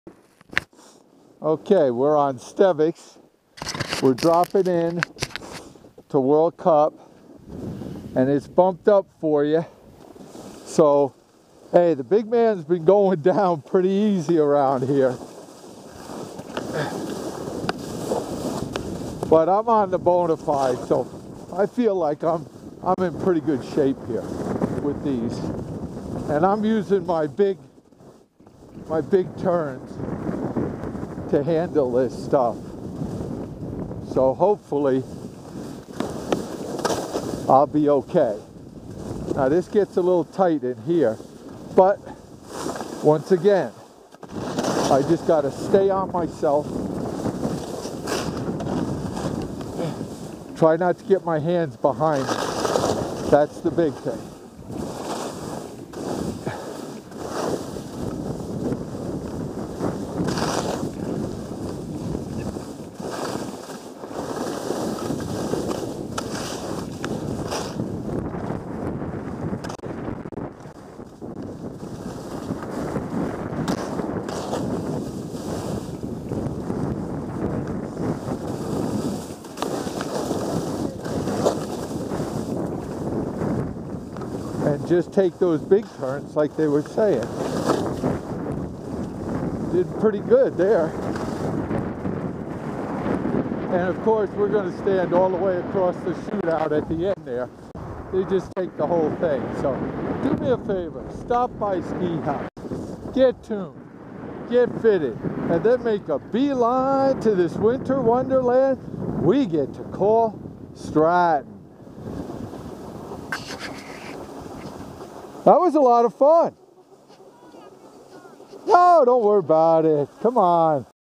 02/10/13 The Big Man smearing the bumps on World Cup.
Blizzard Bonefide smoothing out bumps on SteveK and World Cup.